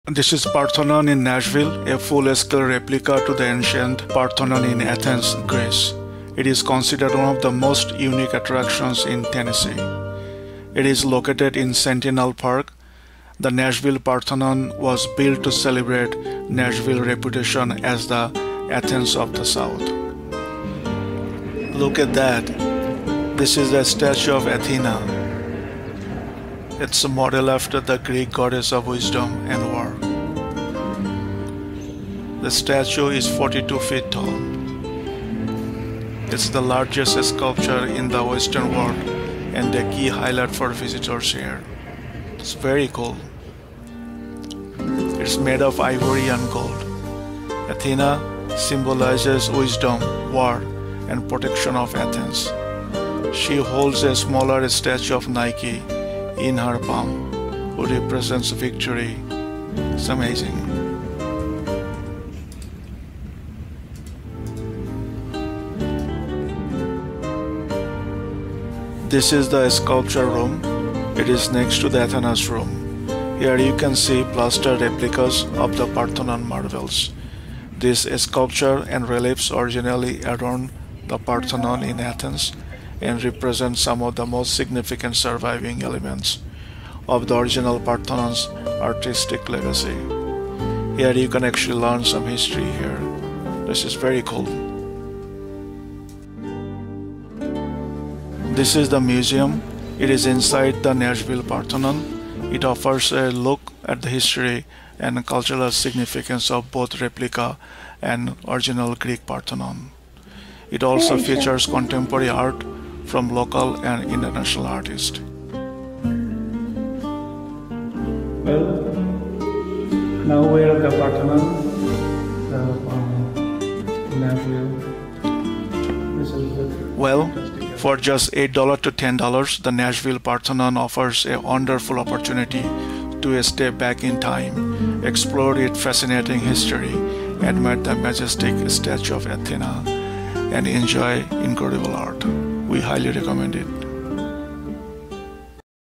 The Parthenon in Nashville: The American Tribute to Greek Glory
🏛️ Explore The Parthenon Nashville, a stunning full-scale replica of Greece's iconic temple! Step inside for art, history, and the 42-ft Athena statue!

Come along as we explore the Parthenon in Nashville, Tennessee, and uncover its unique charm!

🕰️ *TIMESTAMPS: 🕒*

00:00 Outside to Inside the Parthenon
00:21 The Incredible Athena Statue 🗿
01:02 Sculpture Room Highlights
01:42 Art Museum Walkthrough 🖼️

Located in Centennial Park, this attraction offers a glimpse of ancient Greek architecture right here in Tennessee. Inside, we’ll marvel at the impressive 42-foot statue of Athena, made of ivory and gold, explore the Sculpture Room with replicas of the Parthenon Marbles, and browse the art museum. Join us for a short tour through history and culture!

🎟️ *Admission:*
Adults: $10 | Seniors (62+): $8 | Youth (4-17): $8 | Children under 4: Free